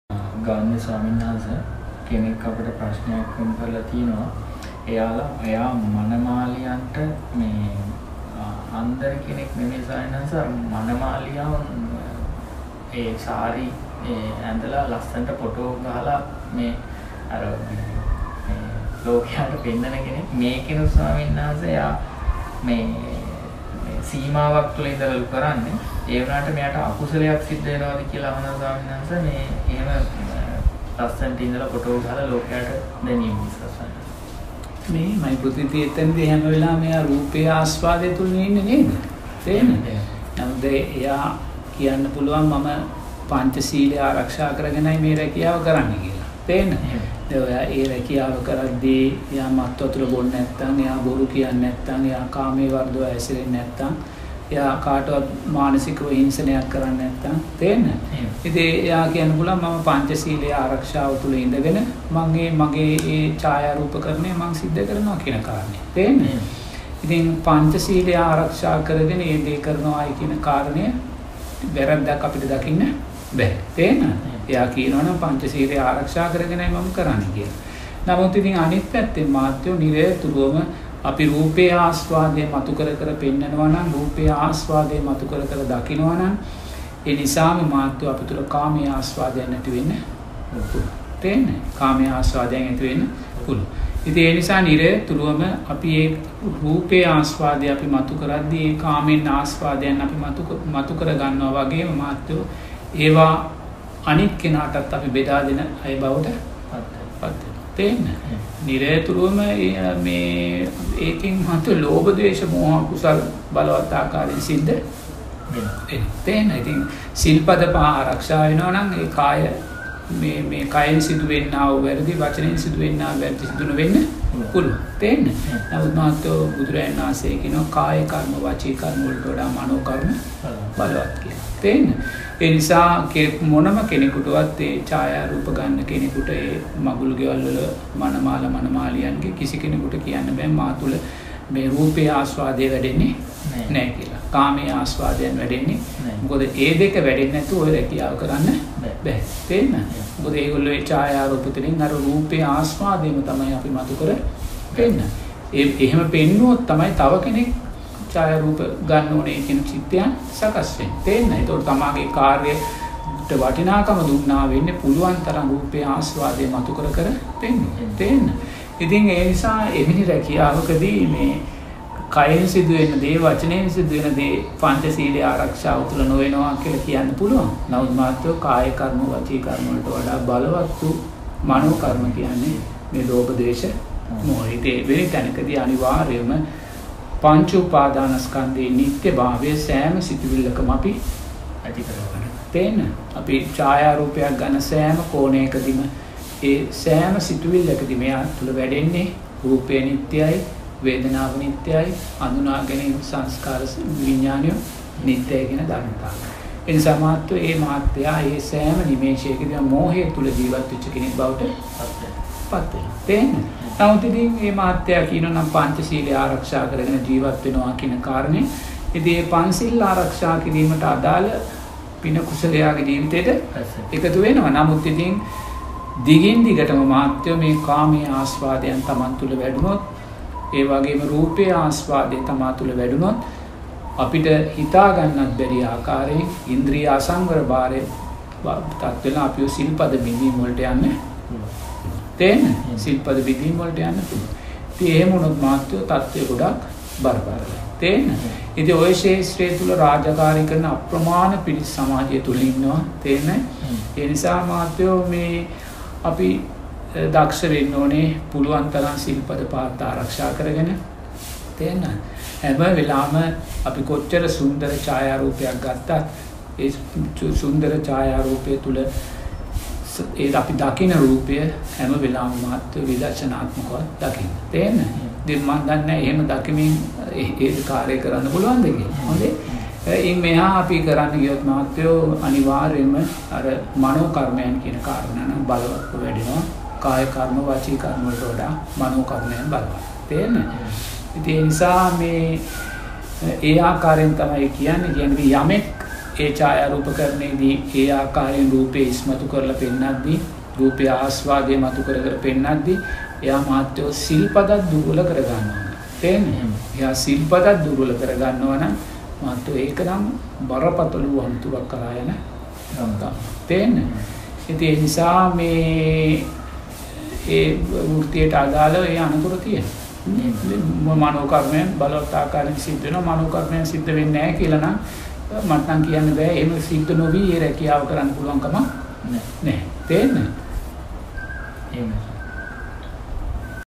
ඡායාරූප ගැනීම නිසා රූපයේ ආශ්වාදය වැඩිවේද? Ven Rajagiriye Ariyagnana swameenwahanse
මේ ඝෝර කටුක සංසාර ගමනෙන් අත්මිදී ශාන්ත අමා නිවනින් සැනසීම උදෙසා මෙය උත්තම පාරමී ධර්මයක්ම වෙිවා!!!